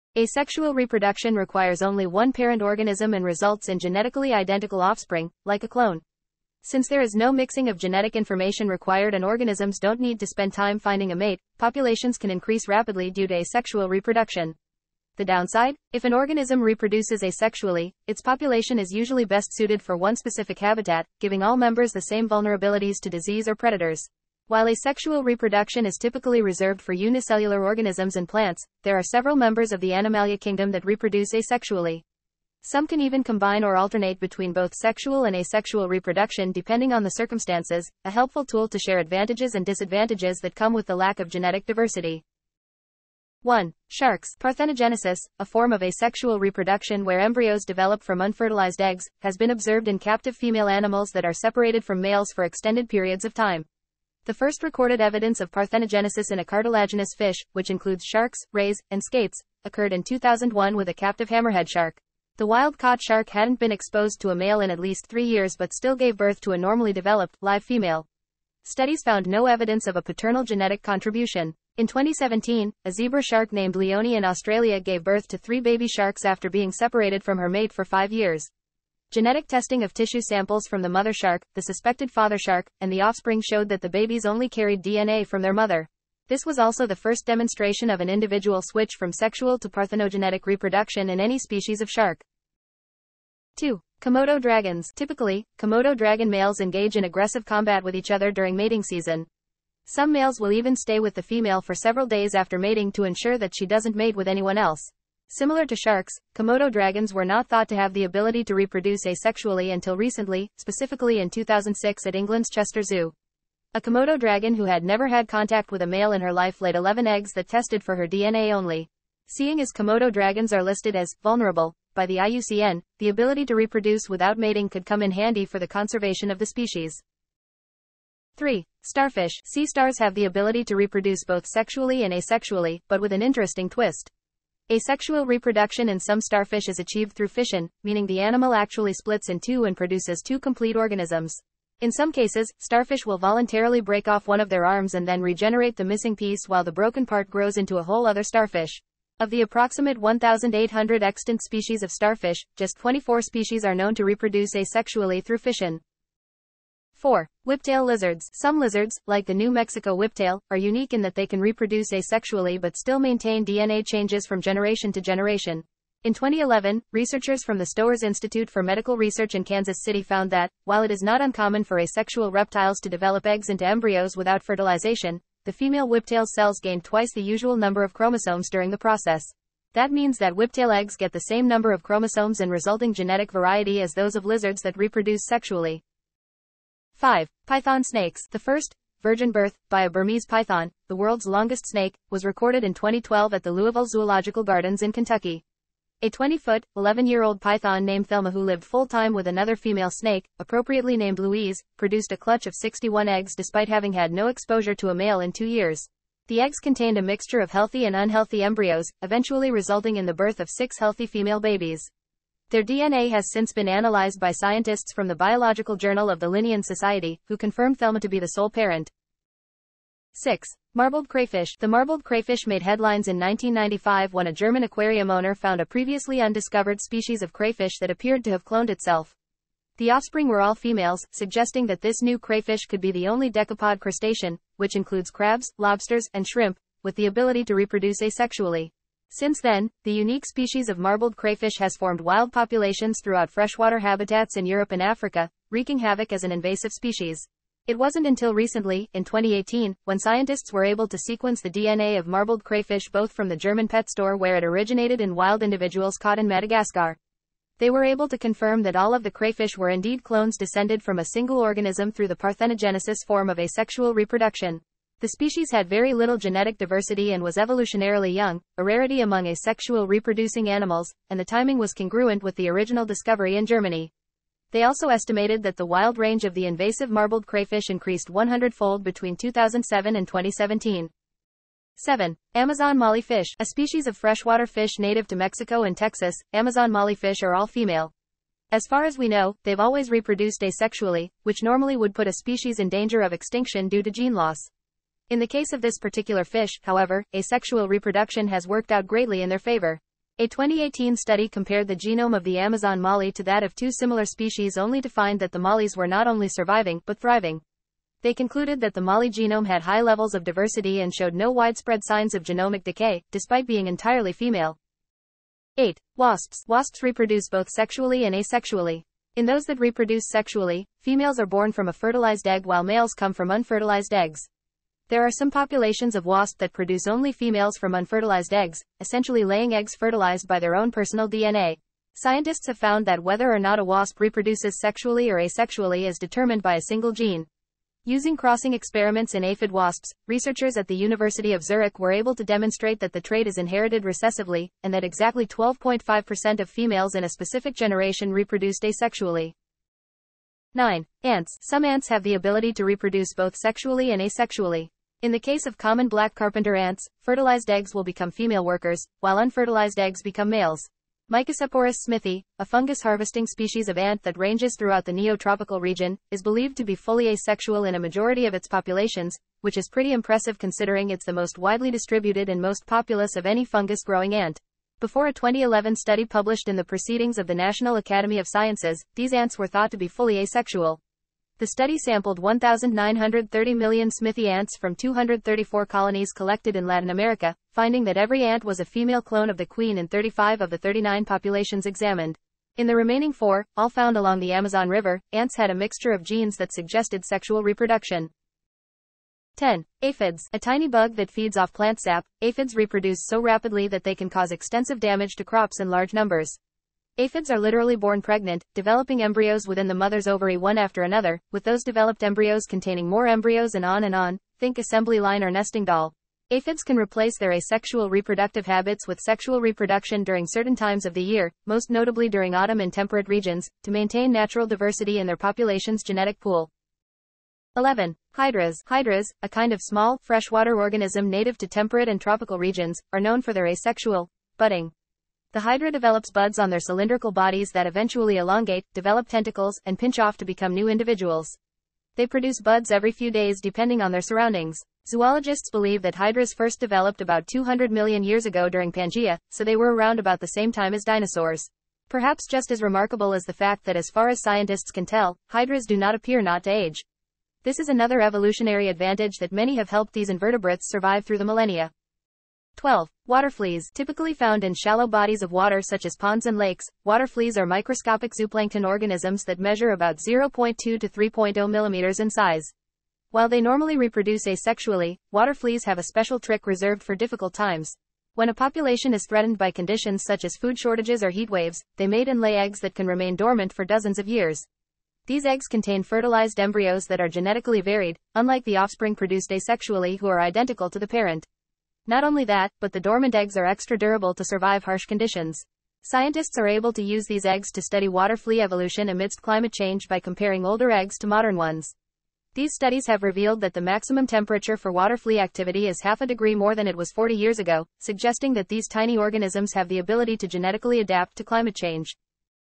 12 Animals That Reproduce Asexually
Asexual reproduction requires only one parent organism and results in genetically identical offspring (like a clone). Since there is no mixing of genetic information required and organisms don’t need to spend time finding a mate, populations can increase rapidly due to asexual reproduction.